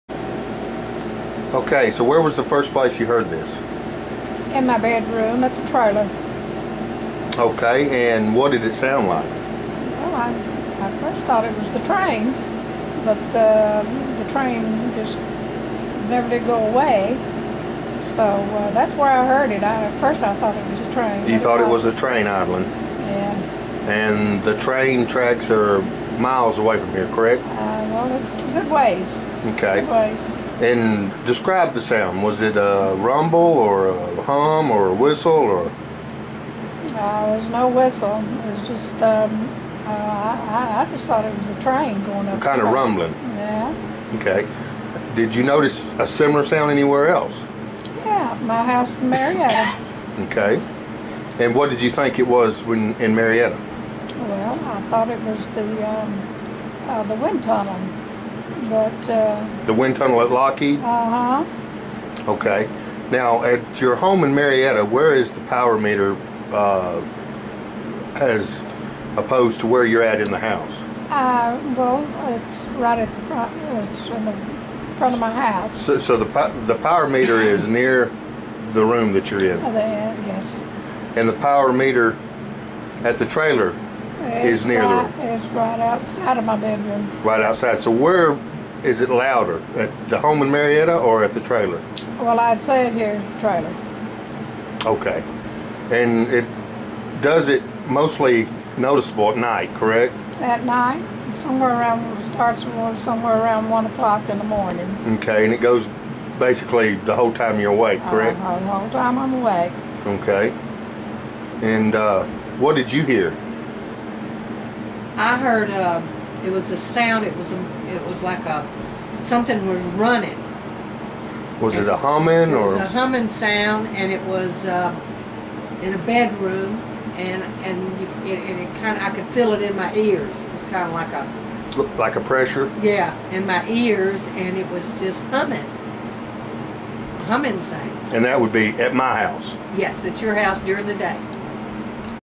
Microwave Frequency Witnesses
2 of 7 confirmed independent witnesses to the RF problems experience on my property in Adairsville, GA.  What they describe is caused by EMF. Very evil stuff, and that is putting it lightly.  They are not actually just hearing what they describe, they are feeling it, and when RF is at this frequency and power, it will eventually kill you. They asked to not be shown on camera. The general reaction when someone describes these types of effects is for someone to say "oh, he's just imagining things" or perhaps schizophrenia. Yeah, right.  That doesn't fly as easily since Havana Syndrome, whose witnesses often describe these same effects, became part of social commentary & intensive investigation.

What witnesses in this video are describing are the use of electro-magnetic weaponry, also known as spectrum or spectral weapons, which use microwaves to harm their targets.  What they are describing are not actually sounds, but the perceived effects of EMF in the microwave band upon water molecules as well as gases within enclosed areas such as pipes and within the inner ear. Like all radio signals, these waves penetrate walls, hard surfaces, and the human body.

These videos were made years before the public heard about Havana Syndrome through the media.  That and /or similar subsets of phenomenon are what is being described here. Cellular radar type devices and tuned masers can cause these disturbances.

I first began mentioning this phenomenon at my residence and the residences of those witnesses in this video in 2009.